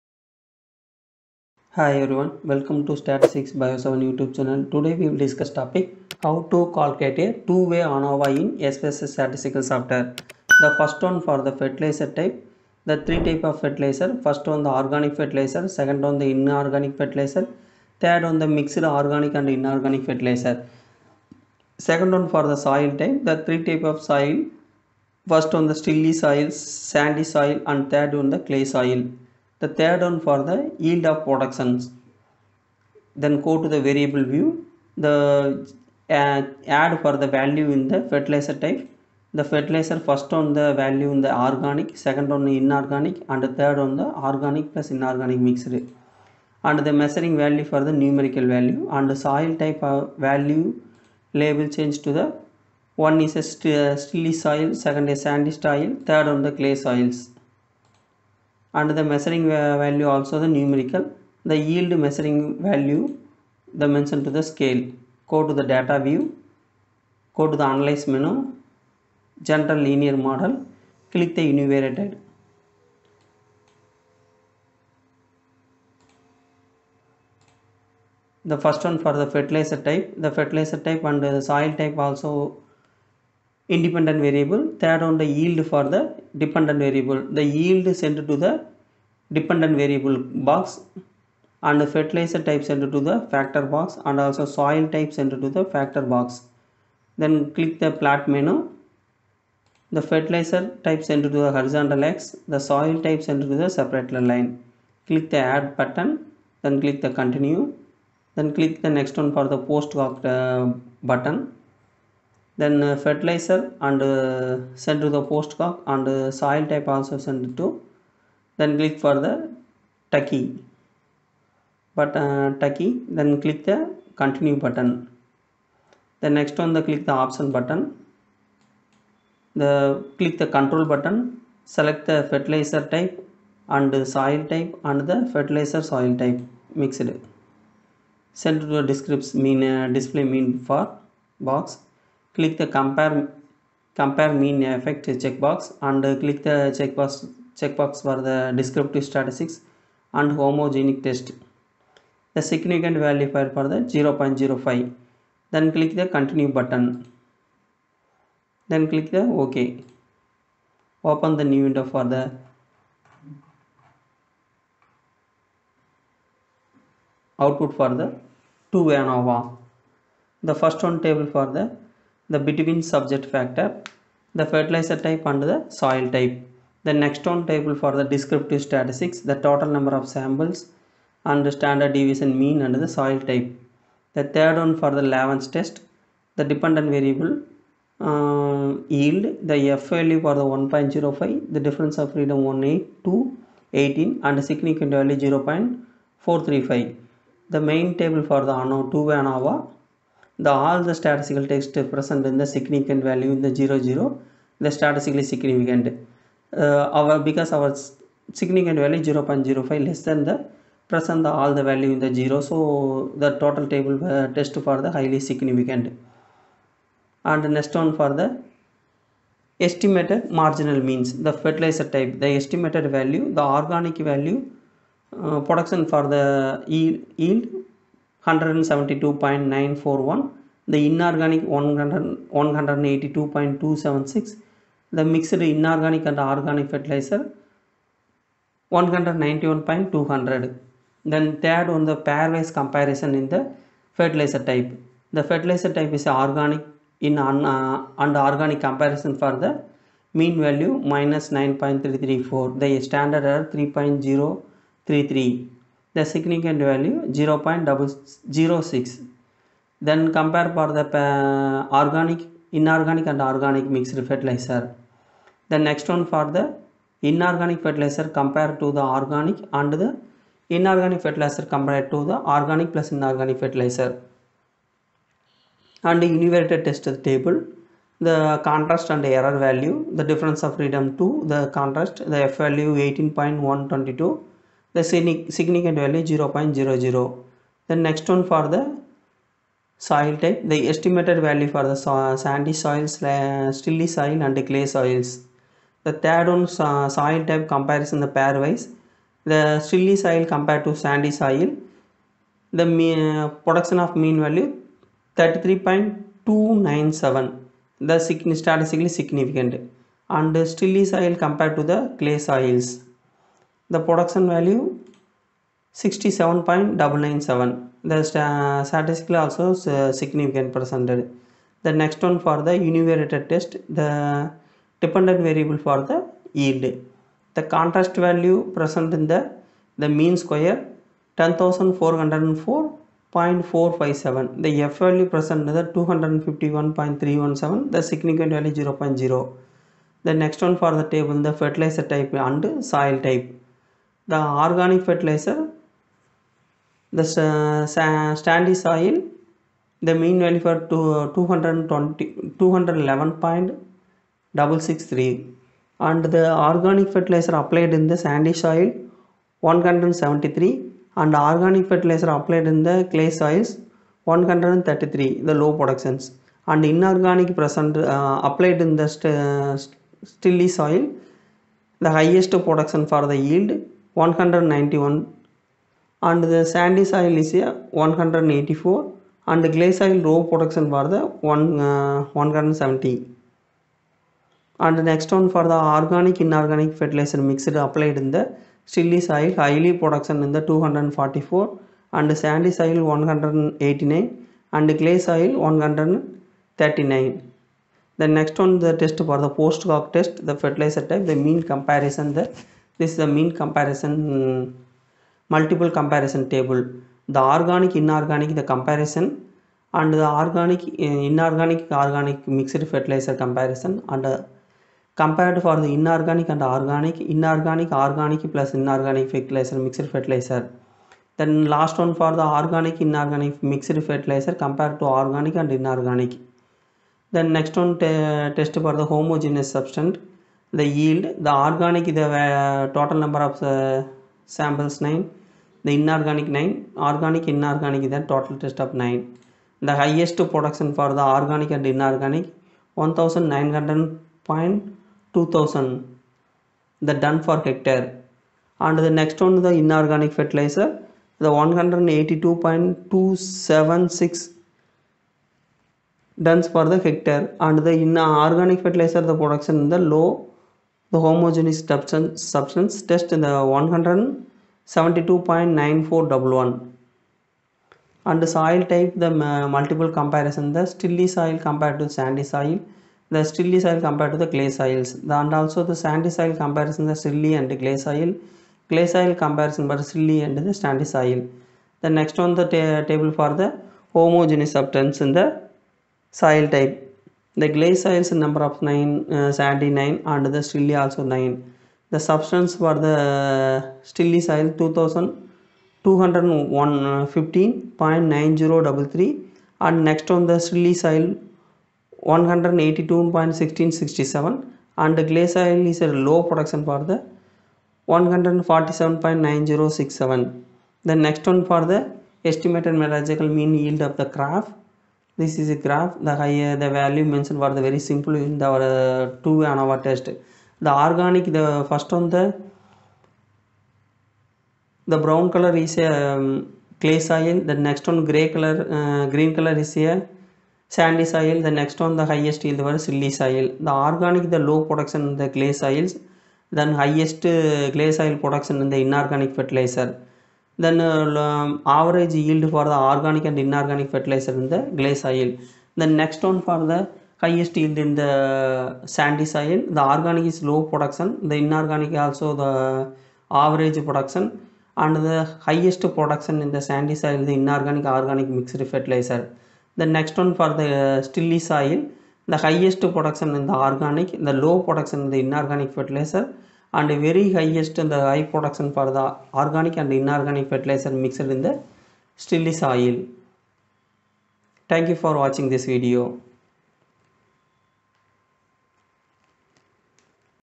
Two-way ANOVA in SPSS Statistics | Analysis of Variance | Post Hoc Tukey Test
In this video tutorial, I will show you How to perform a Two-way ANOVA in SPSS Statistics by using the SPSS 21 version.

Software Version : SPSS 21